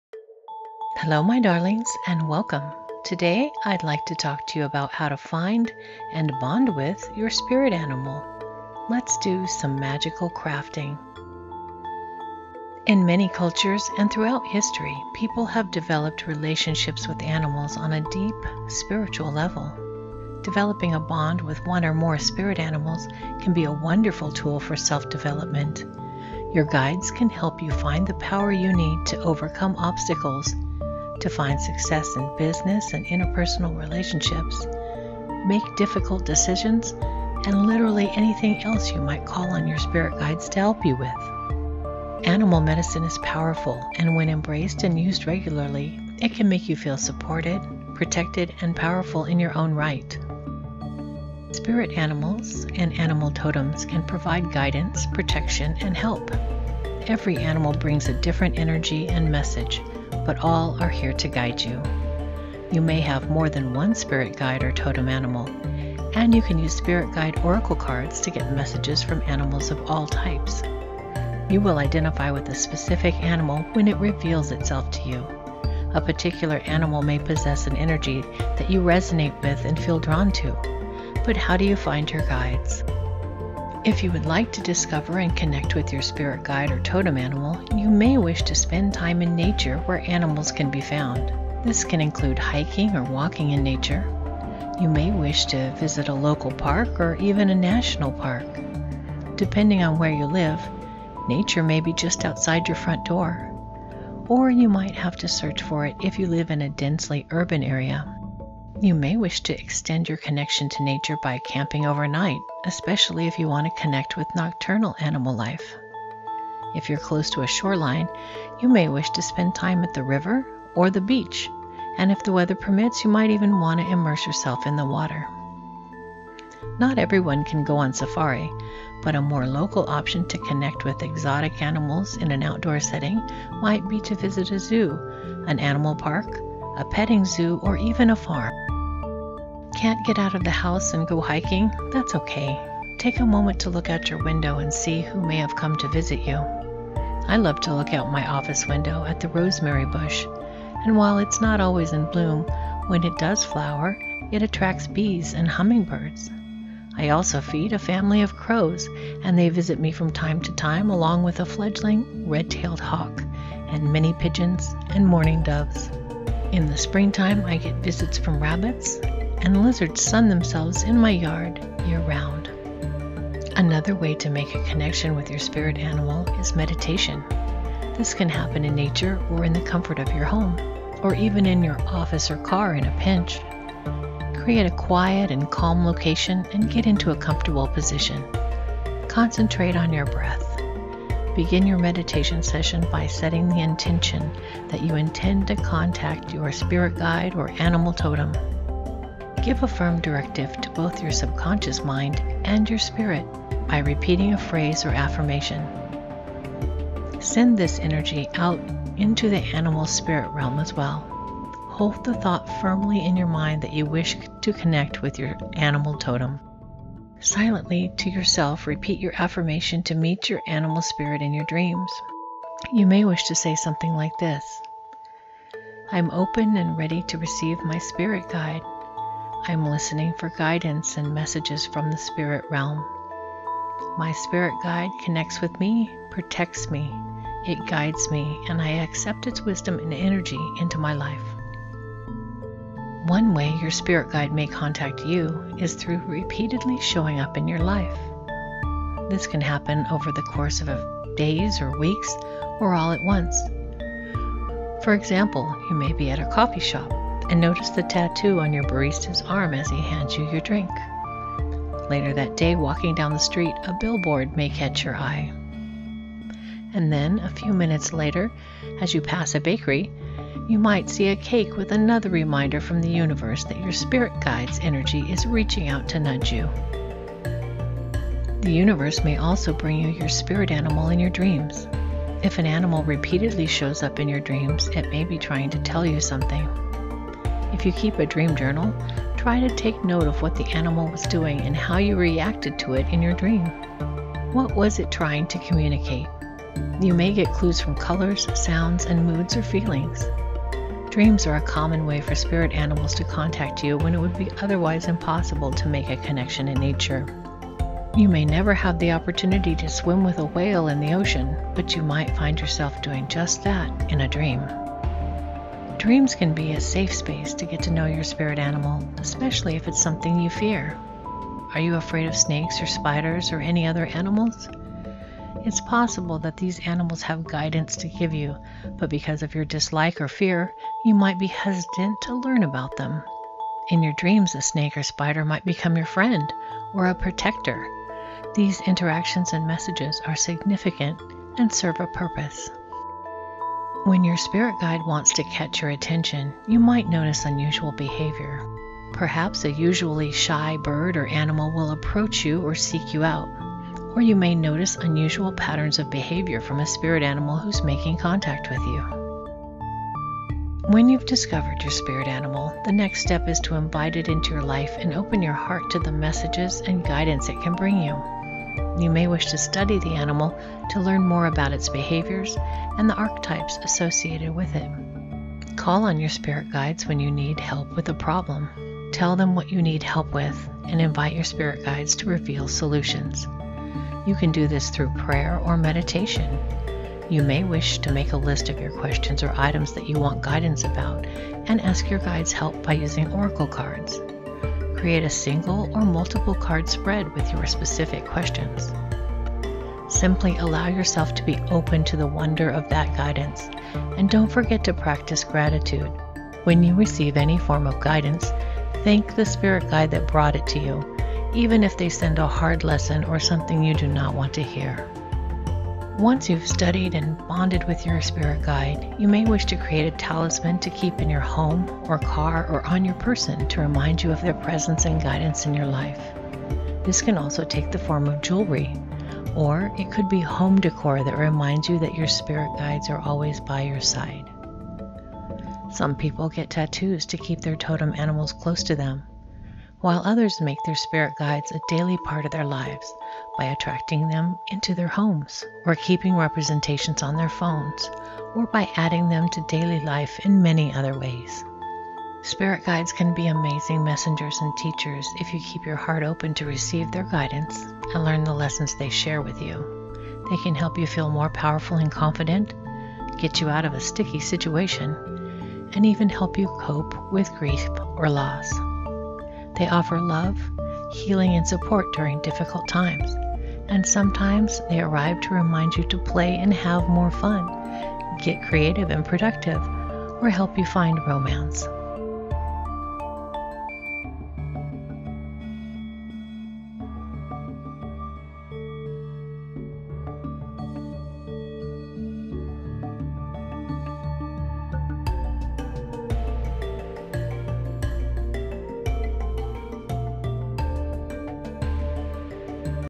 How To Find Your Spirit Guide or Totem Animal - Magical Crafting - Power Animal - Spirit Animal
In this video I talk about how to find and bond with your spirit animals or Totem Animal. Let’s do some magical crafting.
The Ask the Spirit Guides Oracle App is available

🌟 MAGICAL CRAFTING BOOKS
💌 SNAIL MAIL Amy at Magical Crafting 9640 Mission Gorge Rd.

Opening video clip: Videvo, Pexels or Pixabay